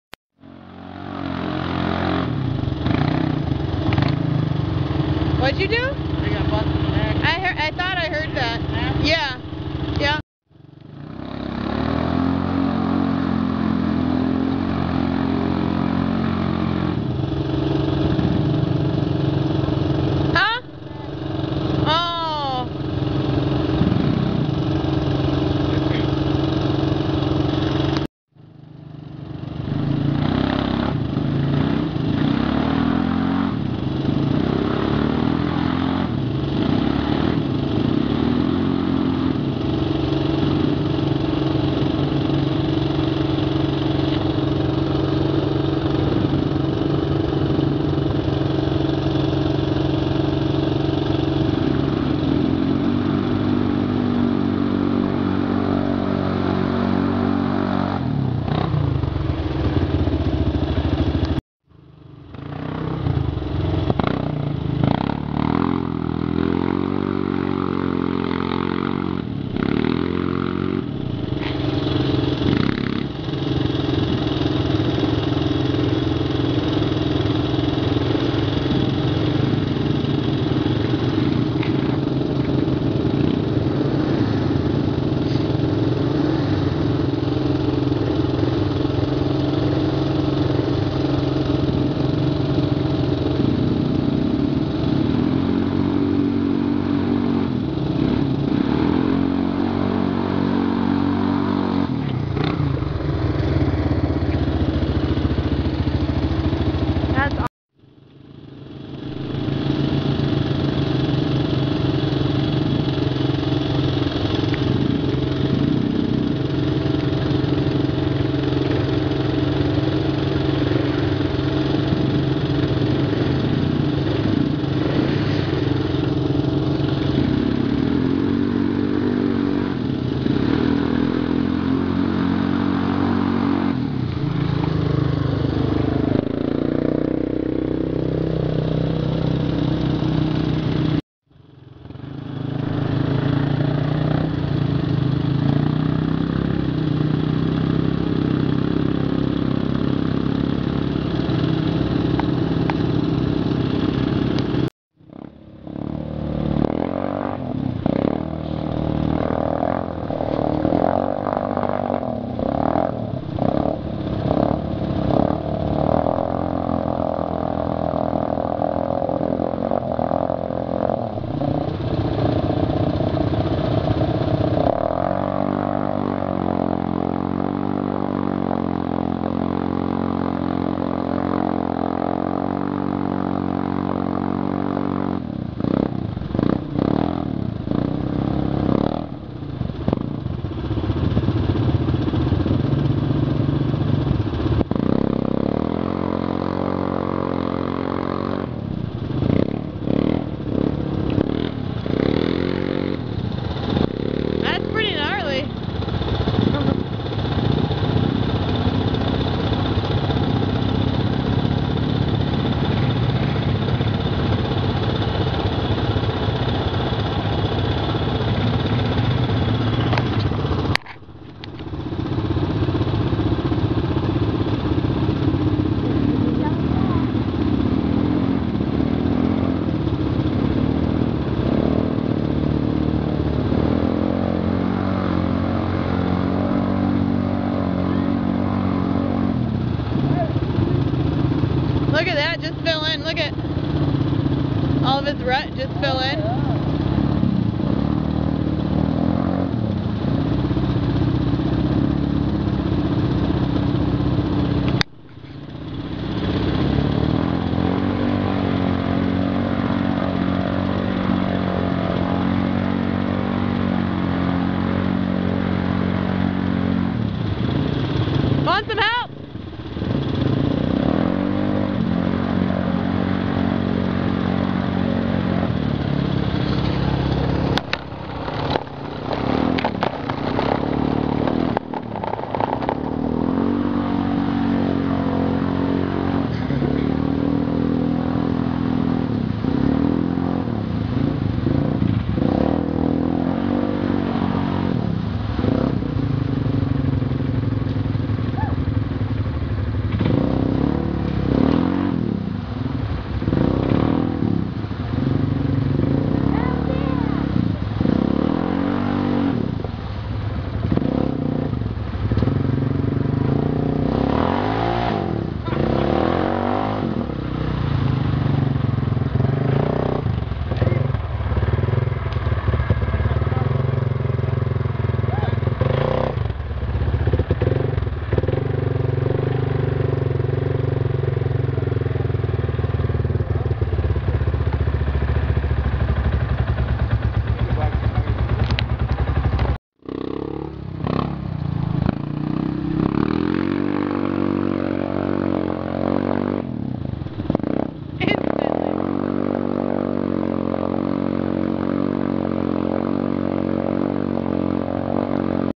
atv going deep
lifted kodiak going deep, broken axle in the fun